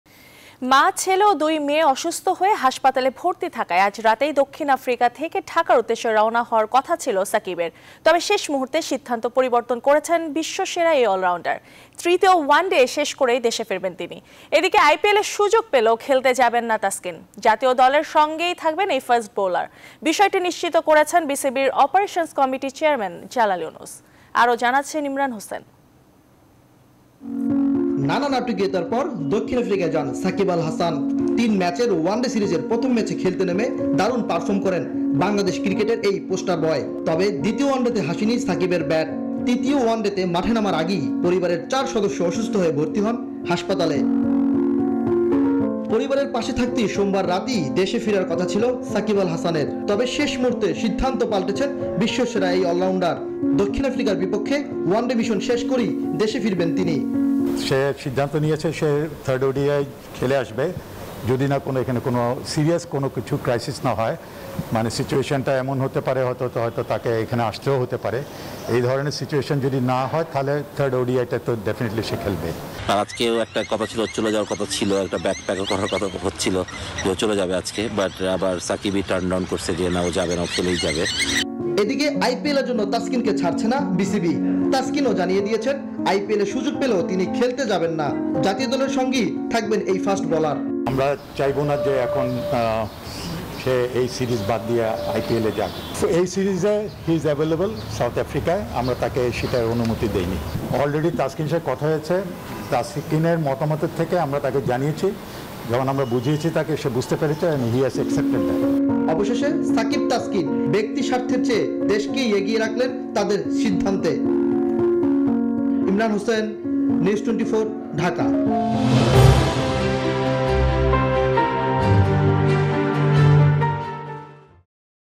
শেষ মুহূর্তে মত পাল্টালেন সাকিব, দক্ষিণ আফ্রিকায় খেলবেন তৃতীয় ওয়ানডেও 21Mar.22| Shakib Al Hasan
মা, ছেলে ও দুই মেয়ে অসুস্থ হয়ে হাসপাতালে ভর্তি থাকায় আজ রাতেই দক্ষিণ আফ্রিকা থেকে ঢাকার উদ্দেশে রওয়া হওয়ার কথা ছিলো সাকিবের। তবে শেষ মুহূর্তে সিদ্ধান্ত পরিবর্তন করছেন বিশ্বসেরা এই অলরাউন্ডার। তৃতীয় ওয়ানডে শেষ করেই দেশে ফিরবেন তিনি। এদিকে, আইপিএলে সুযোগ পেলেও খেলতে যাবেন না তাসকিন। জাতীয় দলের সঙ্গেই থাকবেন এই ফাস্ট বোলার। বিষয়টি নিশ্চিত করেছেন বিসিবির অপরেশন্স কমিটির চেয়ারম্যান জালাল ইউনুস। আরো জানাচ্ছেন ইমরান হোসেন।

নানা নাটকিয়তার পর দক্ষিণ আফ্রিকায় যান সাকিব আল হাসান। তিন ম্যাচের ওয়ানডে সিরিজের প্রথম ম্যাচে খেলতে নেমে দারুণ পারফর্ম করেন বাংলাদেশ ক্রিকেটের পোস্টার বয়। তবে দ্বিতীয় ওয়নাডেতে হাসেনি সাকিবের ব্যাট। তৃতীয় ওয়নাডেতে মাঠে নামার আগেই পরিবারের চার সদস্য অসুস্থ হয়ে ভর্তি হন হাসপাতালে।

পরিবাবারের পাশে থাকতে সোমবার রাতেই দেশে ফেরার কথা ছিলো সাকিব আল হাসানের। তবে শেষ মুহূর্তে সিদ্ধান্ত পাল্টেছেন বিশ্বসেরা এই অলরাউন্ডার। দক্ষিণ আফ্রিকার বিপক্ষে ওয়ানডে সিরিজ শেষ করেই দেশে ফিরবেন তিনি।

এদিকে, আইপিএলের জন্য তাসকিনকে ছাড়ছে না বিসিবি। তাসকিনও জানিয়ে দিয়েছেন, আইপিএলে সুযোগ পেলেও তিনি খেলতে যাবেন না। জাতীয় দলের সঙ্গেই থাকবেন এই ফাস্ট বোলার।

অবশেষে সাকিব-তাসকিন ব্যক্তি স্বার্থের চেয়ে দেশকেই এগিয়ে রাখলেন তাদের সিদ্ধান্তে।

On Aired on NEWS24 on 21st March, 2022
Our other YouTube channels :
News24 Official Address:
NEWS24
371/A
Bashundhara Road
Block – D
Bashundhara Residential Area
Dhaka - 1229